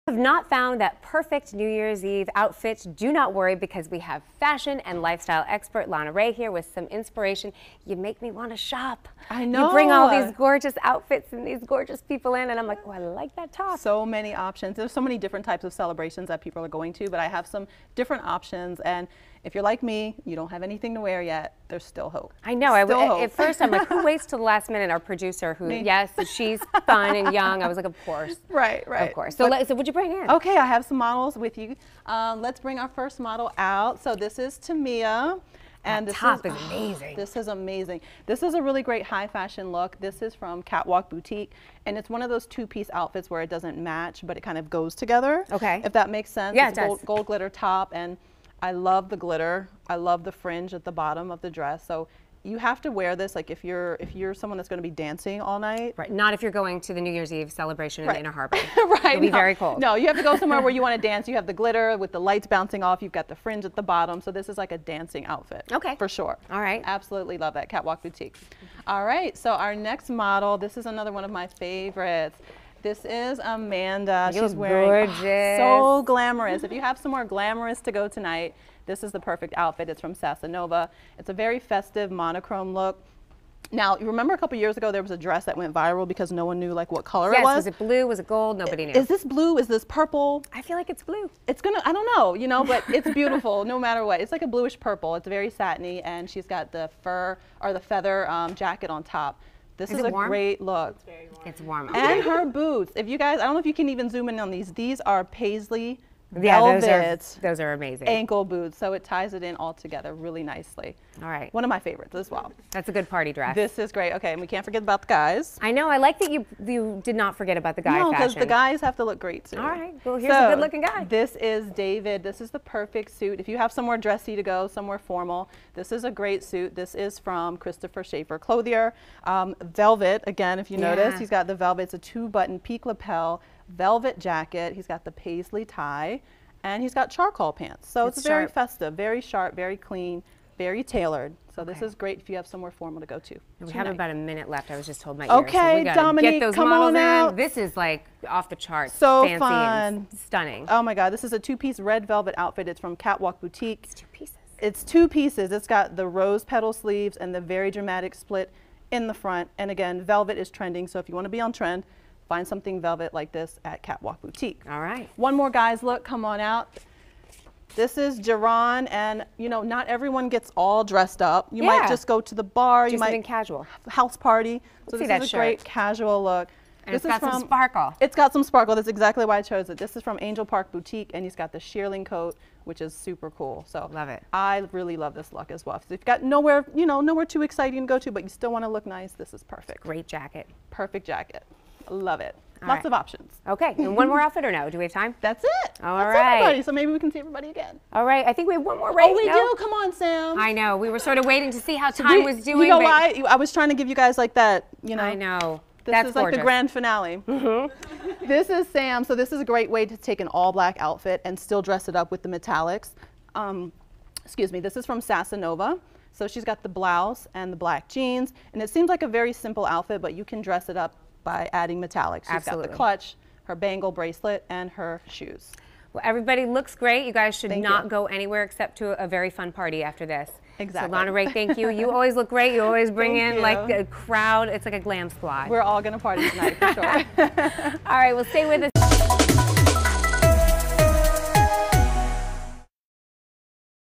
Video: Fashion tips for new year parties
Fashion expert Lana Rae shares some fashion tips for New Year's Eve.